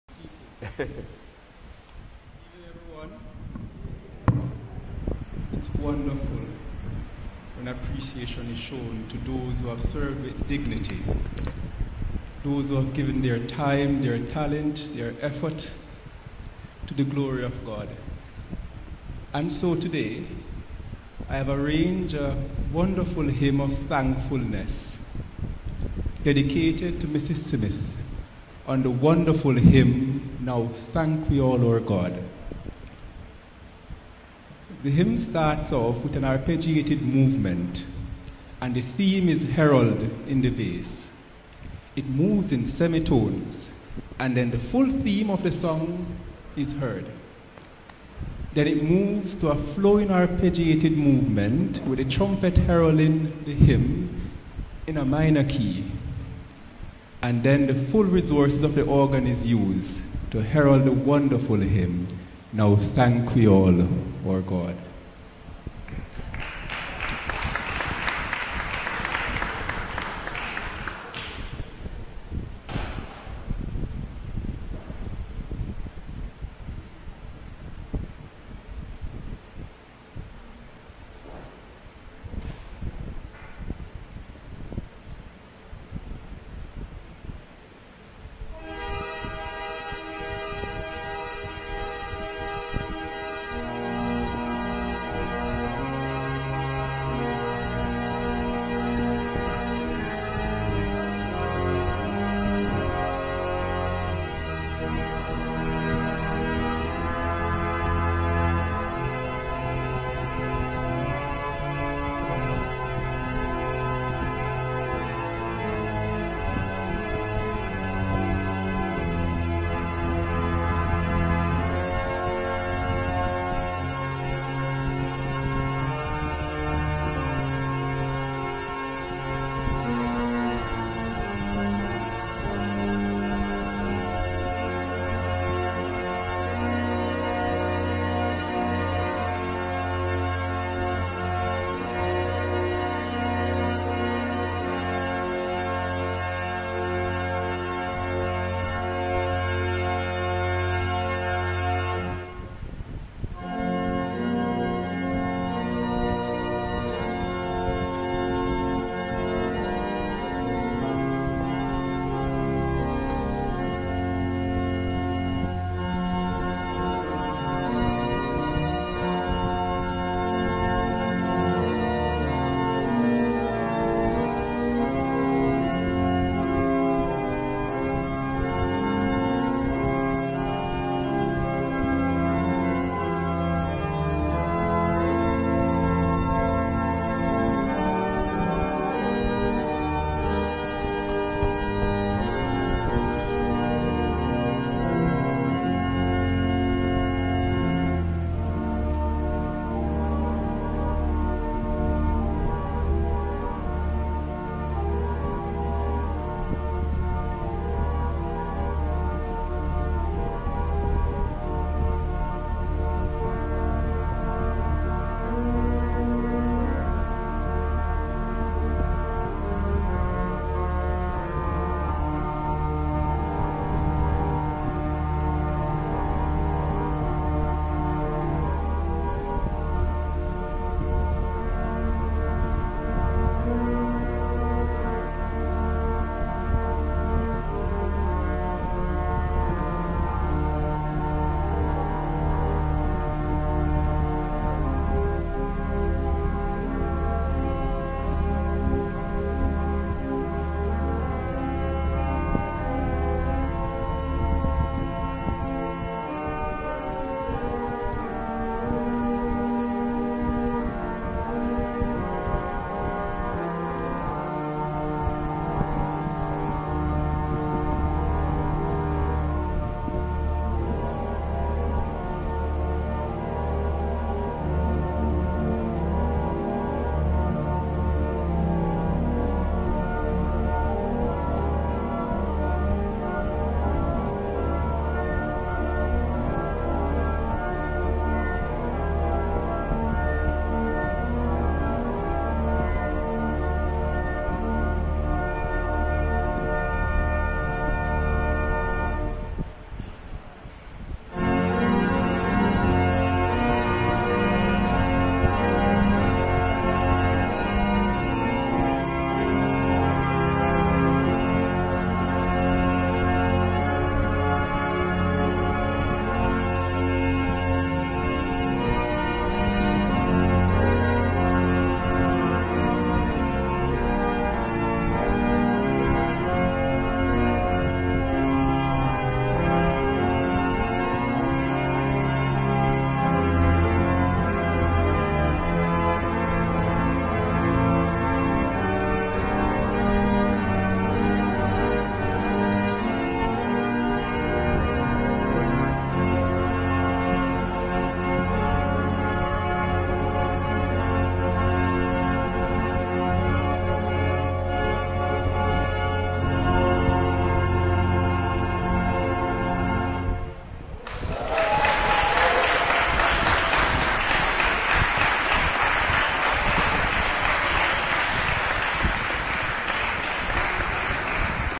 Saint James Church Montago Bay, Jamaica
The historic pipe organ from Henry Bevington, England, from 1890 in the parish church of Saint James, Jamaica.  The organ had been repaired in 2022 but is still not easy to play and several parts of the organ are not functioning. More repairs will fallow.  But the talented organist Alex Gray plays it so well.